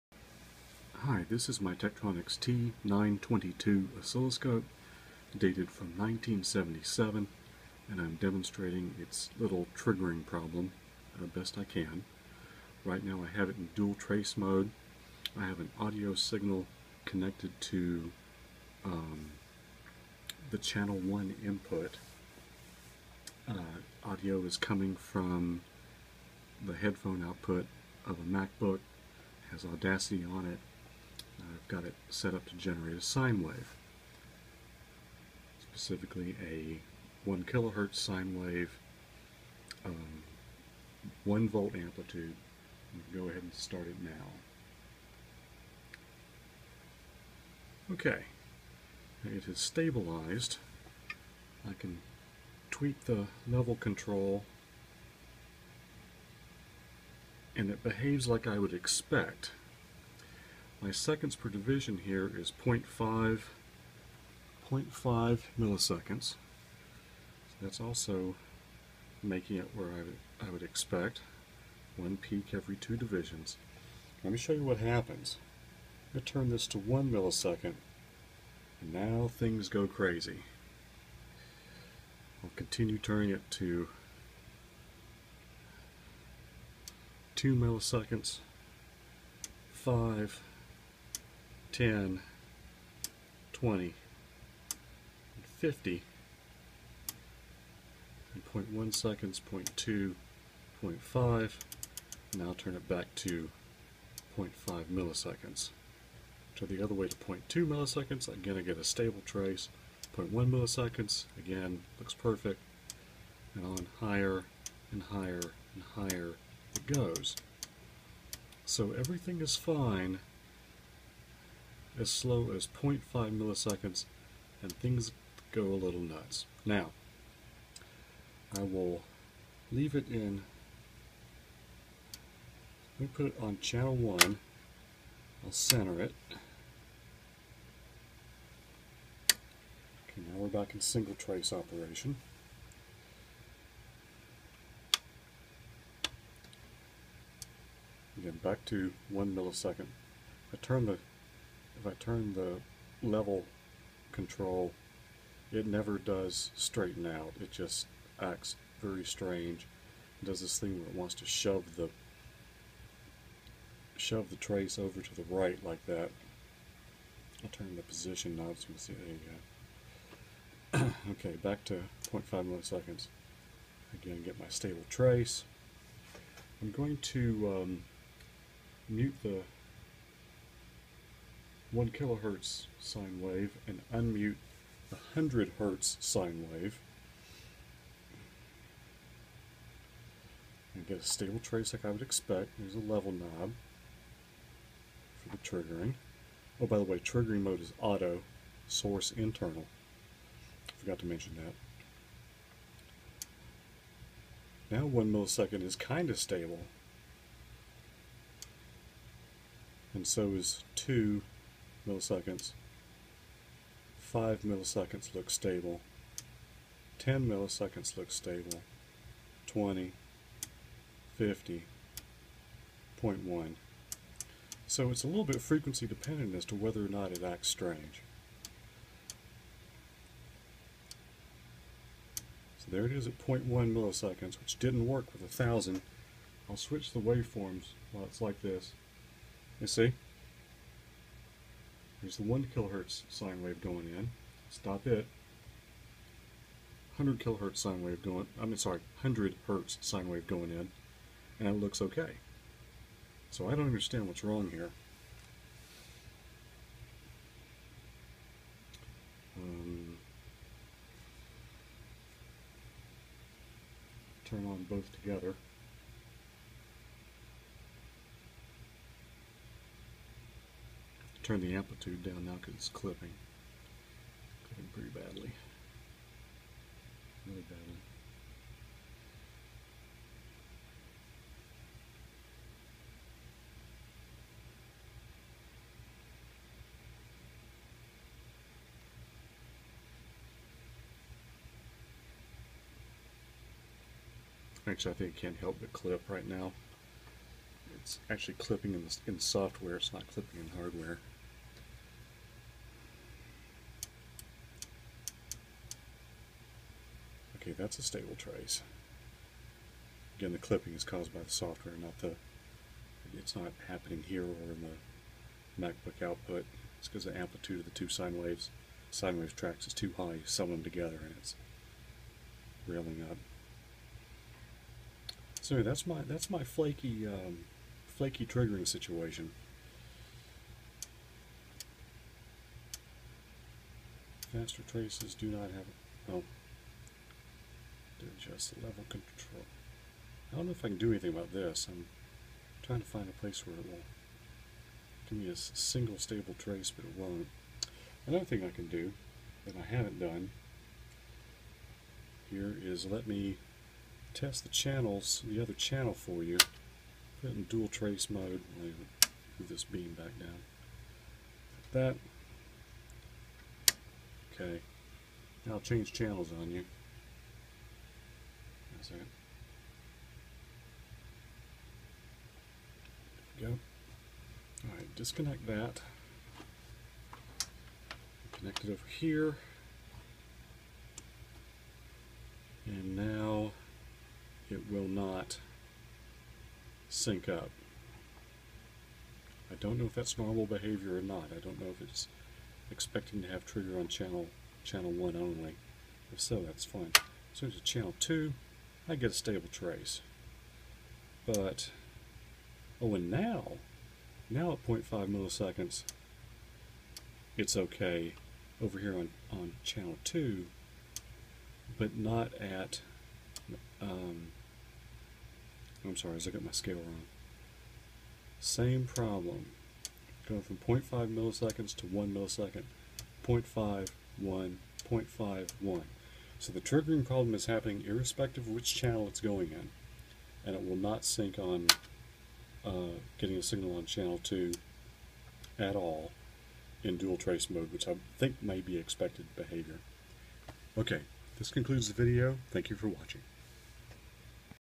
Tektronix T922 with triggering problem
Feeding a 1000Hz and 100Hz sinewave (2V p/p) into an oscilloscope that can't display a stable trace at 0.5ms/div and higher.